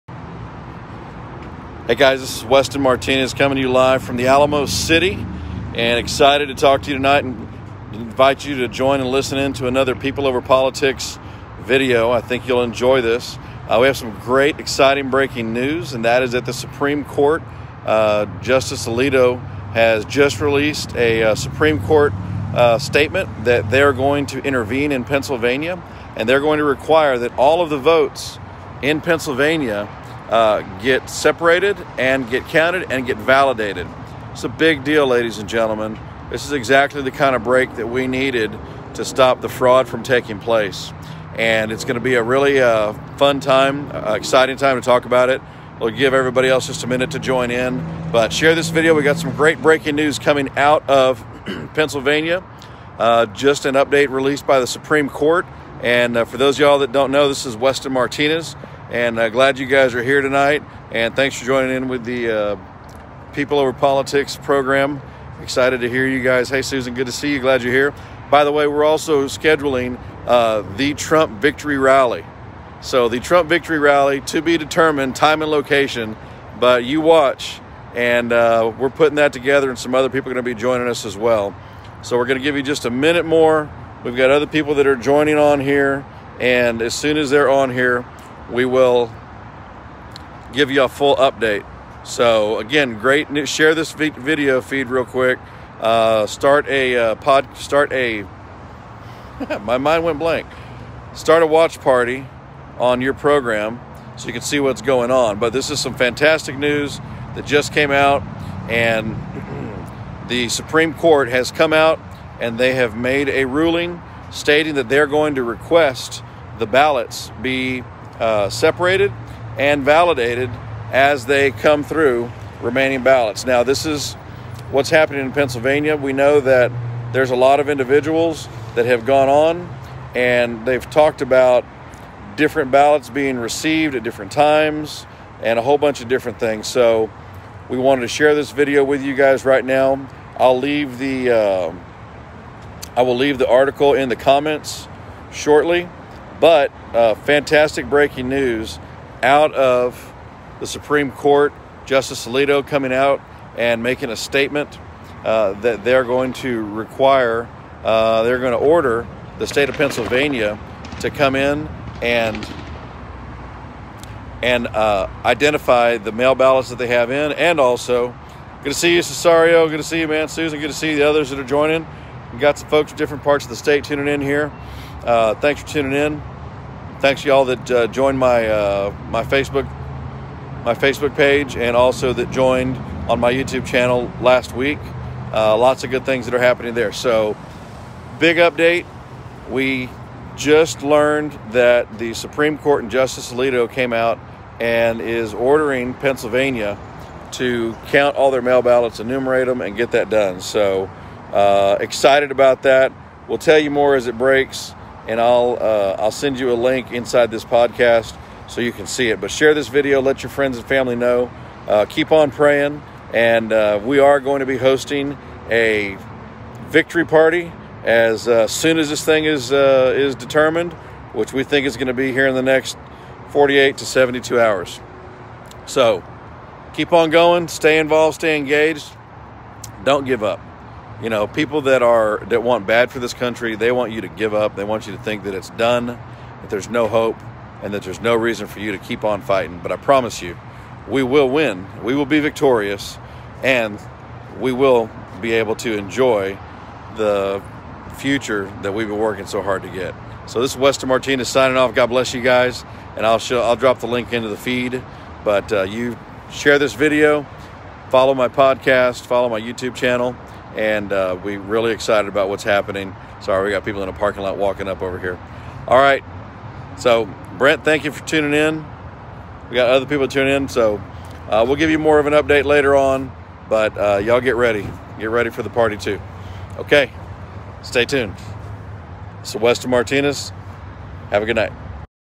Supreme Court Intervenes in Pennsylvania! Requires all ballots be separated and validated!
Supreme Court Intervenes in Pennsylvania!
Requires all ballots be separated and validated! election2020.